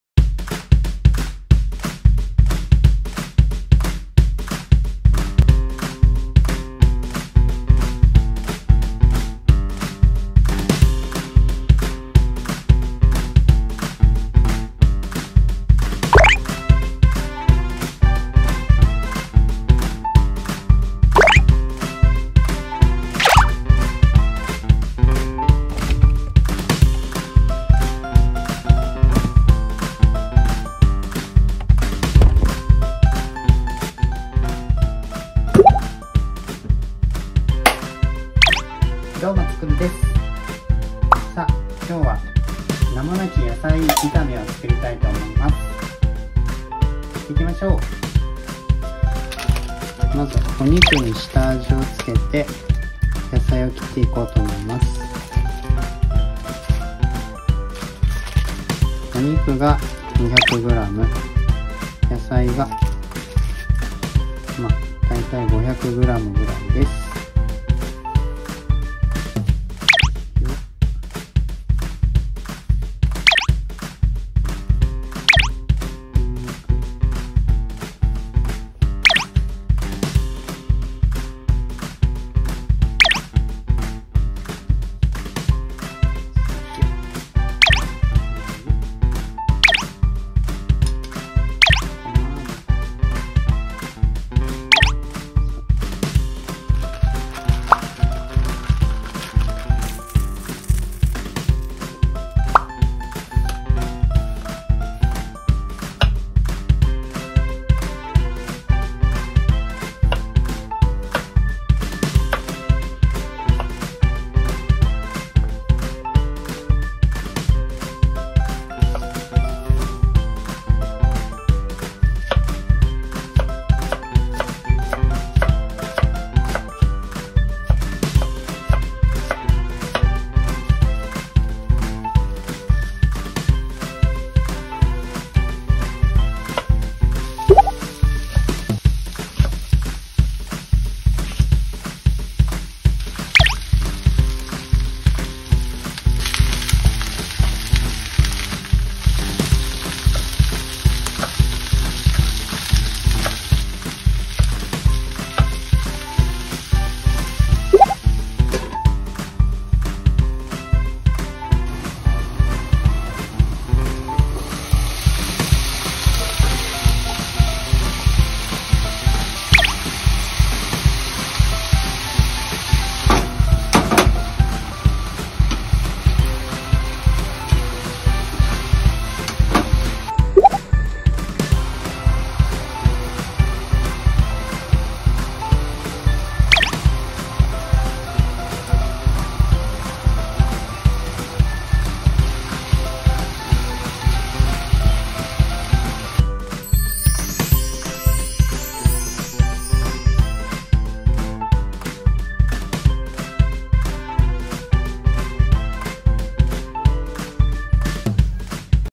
『肉野菜炒め』をつくる！
どうもつくるキッチンです

外出で疲れた日は
簡単に野菜炒めにしましょう

下味をしておくと、お手頃な豚肉も
美味しさアップ

On days when you are tired from going out
 Let's easily stir-fry vegetables

 If you season it, you can also get reasonable pork.
 Deliciousness up